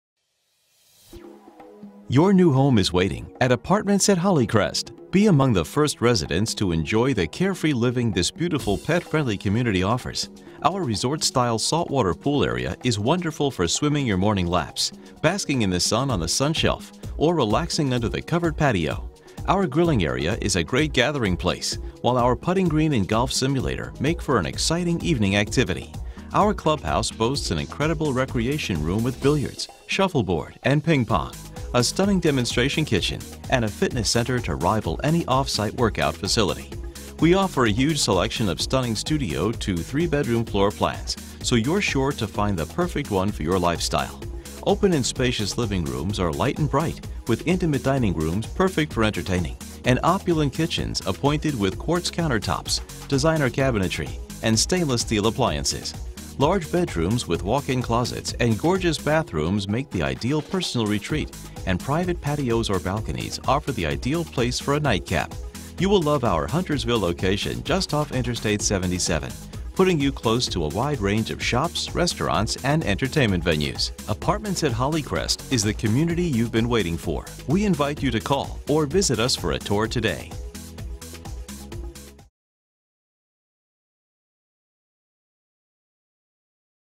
at Holly Crest Apartments in Huntersville, NC - ForRent.com
at Holly Crest Apartments for rent in Huntersville, NC on ForRent.com: Availability, pricing and special promotions subject to change daily. Apartments at Holly Crest offers resort-style living in beautiful studio, one, two and three bedroom luxury apartments.  Each residence is appointed with exclusive amenites throughout such as quartz countertops, stainless appliances, designer cabinetry and much more!  This community redefines luxury apartment living offering 35,000 square feet of outdoor gathering space and 15,000 square feet of indoor amenity space.  You'll love living here where you will enjoy a huge saltwater swimming pool, state-of-the-art fitness center and even a golf simulator with putting green.  Visit today and become one of the first residents at Apartments at Holly Crest! - 16408 Holly Crest Lane